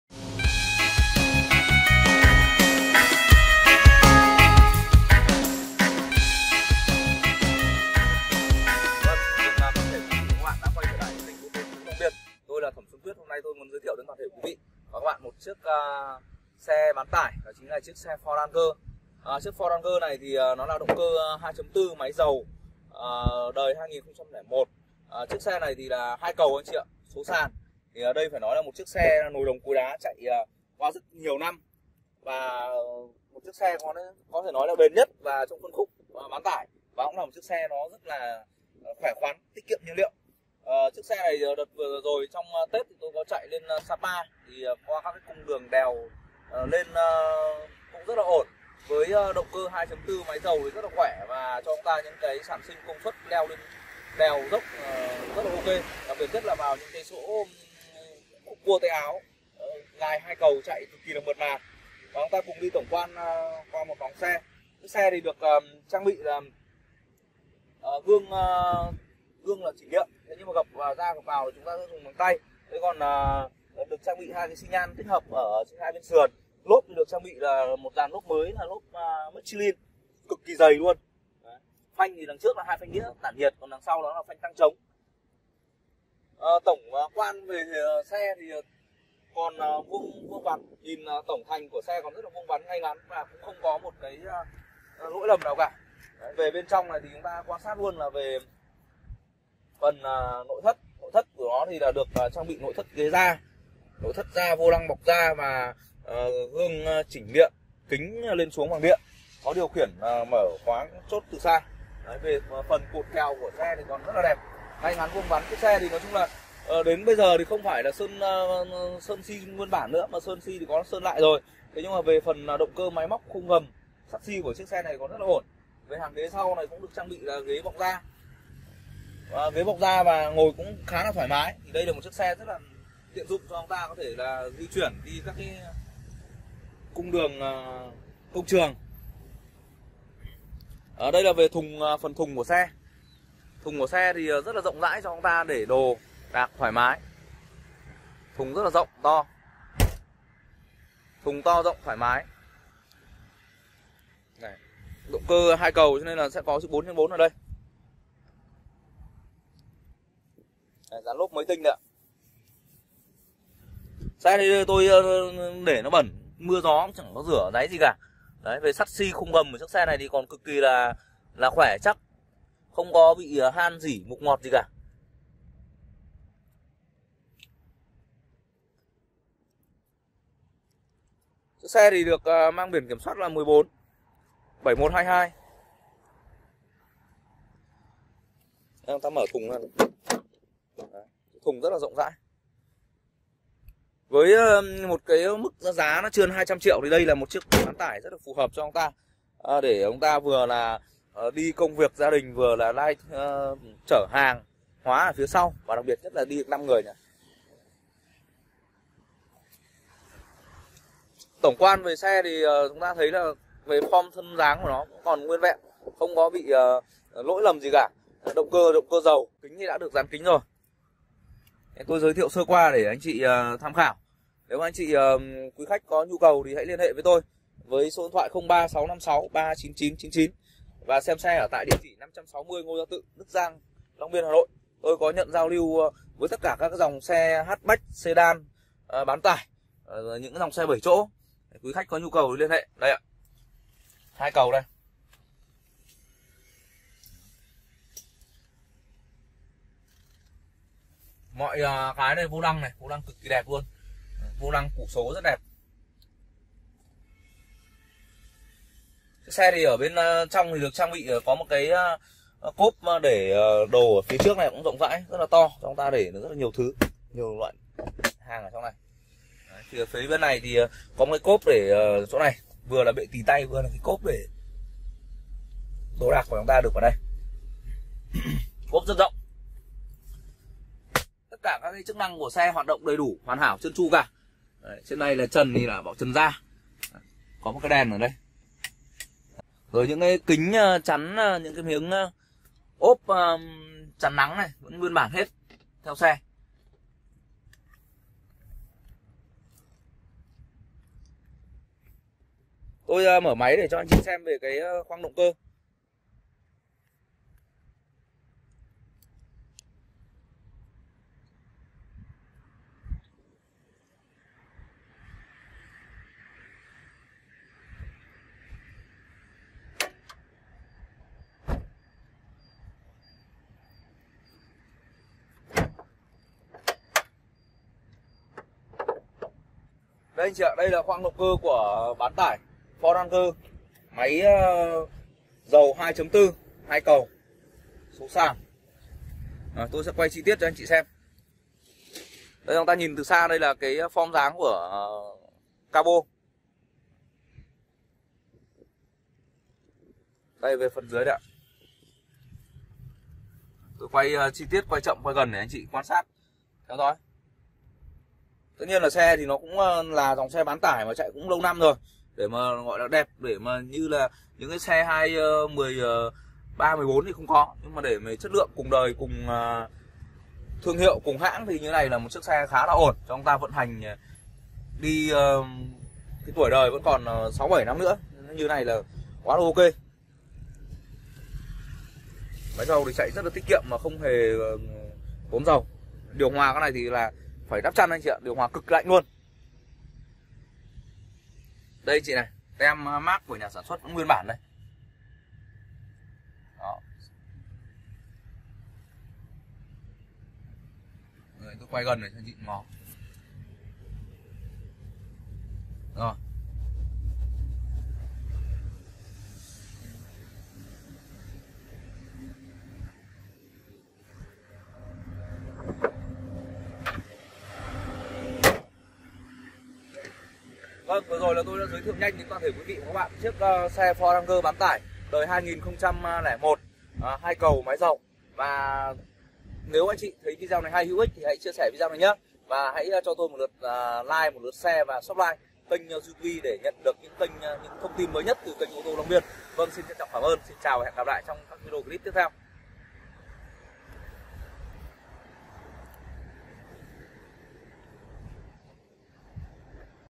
Bán Tải Ford Ranger hai cầu máy dầu số sàn 2 4 0365639999
MẪU XE ĐƯỢC MỆNH DANH LÀ NỒI ĐỒNG CỐI ĐÁ.
Dòng Xe Bán tải được mệnh danh là Vua của các dòng xe bán tải.
Ford Ranger nghe thôi đã thấy giá trị của thương hiệu này mang lại rồi,
Ford Ranger mẫu xe tiện ích thông dụng rất phổ thông tại thị trường Châu Á, và đặc biệt tại VN,
Xe được trang bị động cơ 4 máy 2.4. nhiên liệu dầu, hộp số sàn, hai cầu.
Vô lăng điều chỉnh hai hướng lên xuống, trợ lái dầu, phanh đĩa tản nhiệt.gương điện , kính điện, nội thất ghế da màu kem.5 chỗ ngồi thuận tiện di chuyển các cung đường xa.
Quý khách xem xe và lái thử tại 560 Ngô Gia Tư Long Biên HN.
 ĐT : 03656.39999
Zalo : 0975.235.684